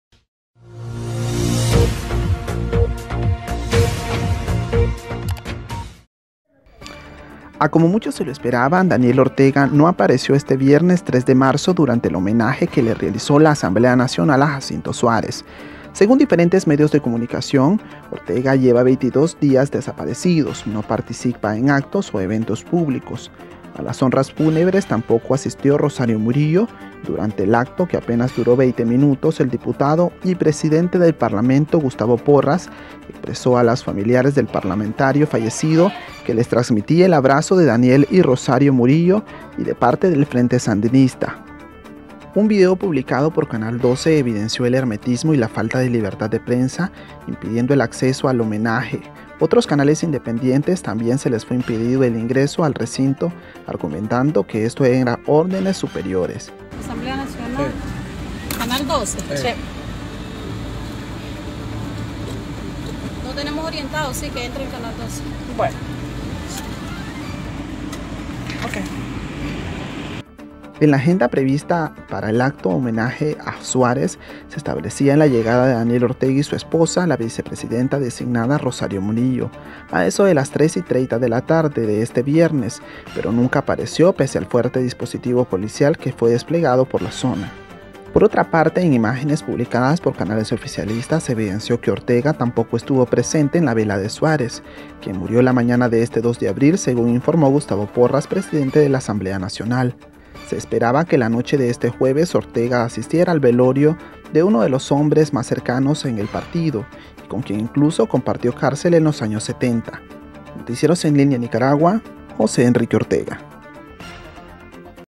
Daniel Ortega no asistió a la vela ni homenaje de su amigo Jacinto Suarez | NELN
A como muchos se lo esperaban Daniel Ortega no apareció este viernes 3 de marzo durante el homenaje que realizo la Asamblea Nacional a Jacinto Suárez.
Según diferentes medios de comunicación, Ortega lleva 22 días desaparecidos, no participa en actos o eventos públicos.
A las honras fúnebres tampoco asistió Rosario Murillo. Durante el acto a apenas duro 20 minutos el diputado presidente del parlamento  Gustavo Porras, expresó a los familiares del parlamentario fallecido que les «transmitía el abrazo de Daniel y de Rosario y del Frente Sandinista»